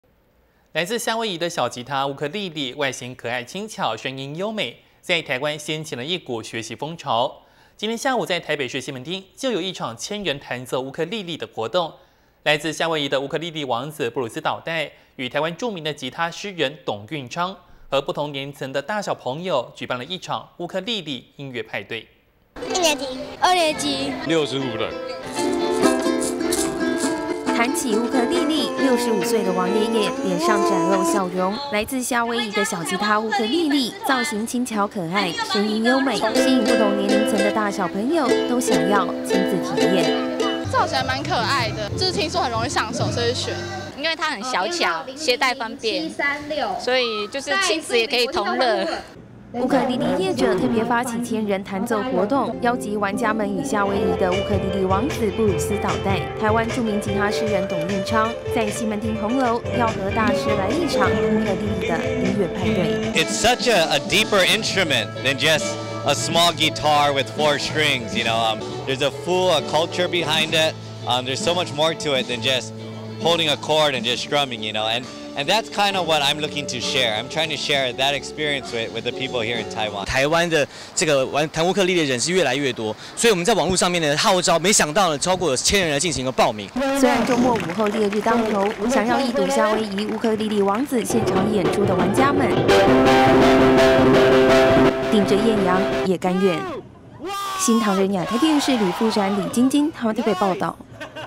【娛樂新聞_烏克麗麗】千人彈奏烏克麗麗 引爆西門町紅樓
【娛樂新聞_烏克麗麗】千人彈奏烏克麗麗 引爆西門町紅樓(NTDAPTV)
來自夏威夷的小吉他烏克麗麗，外型可愛輕巧，聲音優美，在台灣掀起一股學習風潮。今天(20日)下午在台北市西門町，就有一場千人彈奏烏克麗麗的活動，來自夏威夷的烏克麗麗王子布魯斯‧島袋與台灣著名吉他詩人董運昌，和不同年齡層的大小朋友，舉辦一場烏克麗麗的音樂派對。【新唐人亞太台2012年10月20日訊】

烏克麗麗玩家：「一年級。」

烏克麗麗玩家：「二年級。」

烏克麗麗玩家王先生：「65(歲)了。」

彈起烏克麗麗，65歲的王爺爺，臉上展露笑容，來自夏威夷的小吉他 烏克麗麗，造型輕巧可愛，聲音優美，吸引不同年齡層的大小朋友，都想要親自體驗。

烏克麗麗玩家陳同學：「造型還滿可愛的，就聽說很容易上手，所以學。」

烏克麗麗玩家余小姐：「因為它很小巧，攜帶方便，所以就是親子也可以同樂。」

烏克麗麗業者特別發起千人彈奏活動，邀集玩家們與夏威夷的烏克麗麗王子布魯斯‧島袋(Bruce Shimabukuro)、台灣著名吉他詩人董運昌，在西門町紅樓，要和大師來一場烏克麗麗的音樂派對。

烏克麗麗演奏家布魯斯‧島袋：「這是一個很有深度的樂器，它不僅是只有四個弦的小吉他。它還包含了很多的文化，這也就是我試著想要分享，給台灣玩家的精神。」

活動主辦單位發言人石宗仁：「台灣這個彈烏克麗麗的人，是越來越多，所以我們在網路上面號召，沒想到超過千人來報名。」

雖然週末午後烈日當頭，想要一睹夏威夷烏克麗麗王子現場演出的玩家們，頂著豔陽也甘願。